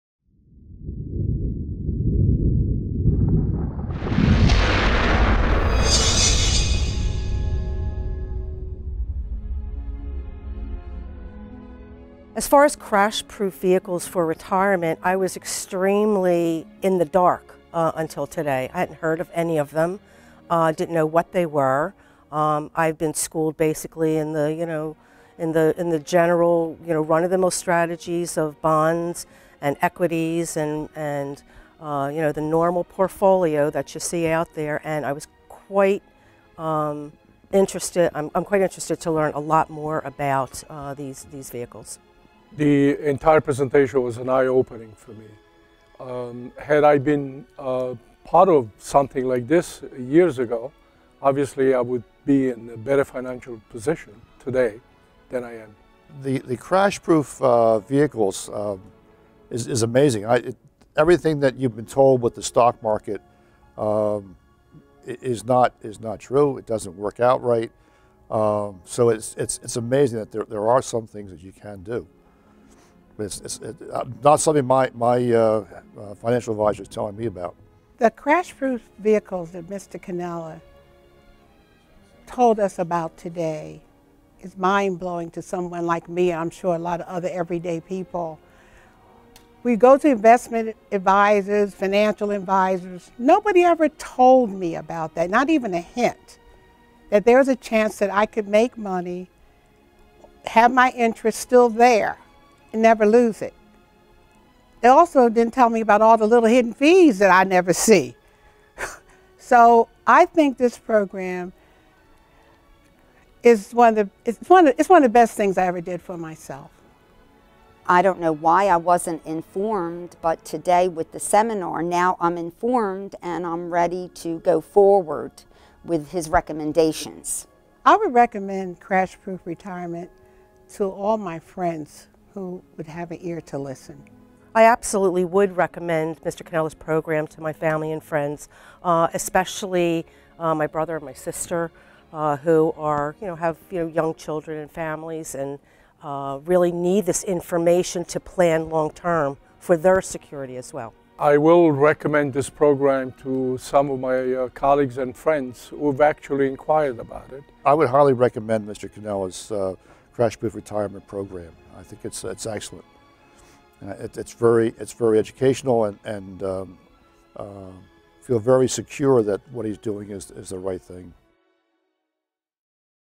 A Crash Proof Retirement Educational Event - Springfield CC
Each week Phil Cannella and Joann Small invite Americans in or near retirement to learn about the Crash Proof alternatives that are outside of the securities industry and devoid of market risk or market fees.

"As far as Crash Proof vehicles for retirement I was extremely in the dark until today I hadn't heard of any of them I didn't know what they were I've been schooled basically in the, you know, in the in the general run of the mill strategies of bonds and equities and and you know the normal portfolio that you see out there and I was quite interested. I'm quite interested to learn a lot more about these these vehicles."

"The entire presentation was an eye-opening for me had I been a part of something like this years ago obviously I would be in a better financial position today then I am."

"The Crash Proof vehicles are amazing. Everything that you've been told with the stock market is not is not true it doesn't work out right, so it's amazing that there are some things that you can do. Not something my financial advisor is telling me about."

"The Crash Proof vehicles that Mr. Cannella told us about today is mind-blowing to someone like me i'm sure a lot of other everyday people we go to investment advisors financial advisors nobody ever told me about that not even a hint that there's a chance that i could make money, have my interest still there and never lose it it also didn't tell me about all the little hidden fees that i never see. So I think this program is one of the best things I ever did for myself."

"I don't know why I wasn't informed but today with the seminar now I'm informed and I'm ready to go forward with his recommendations."

"I would recommend Crash Proof Retirement to all my friends who would have an ear to listen."

"I absolutely would recommend Mr. Cannella's program to my family and friends especially my brother or my sister who are you know have young children and families and really need this information to plan long-term for their security as well."

"I will recommend this program to some of my colleagues and friends who have actually inquired about it."

"I would highly recommend Mr. Cannella's Crash Proof Retirement program I think it's excellent. It's very educational and feel very secure that what he's doing is the right thing."